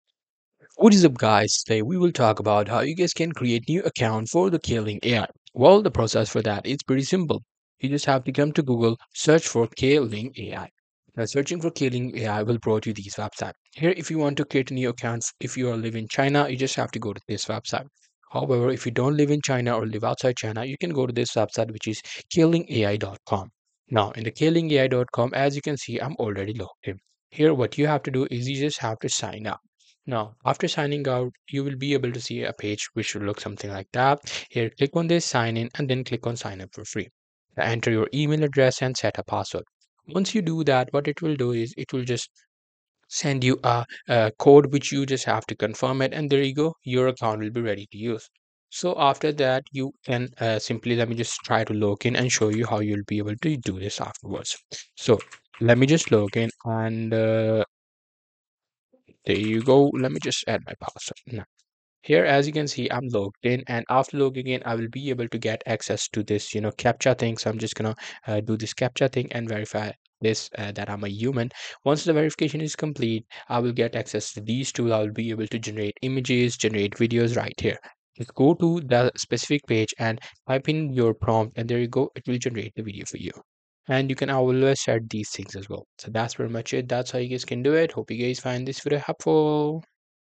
How To Create New Account Kling AI (2024) - FULL GUIDE!
In this video I will show you how to create new account kling ai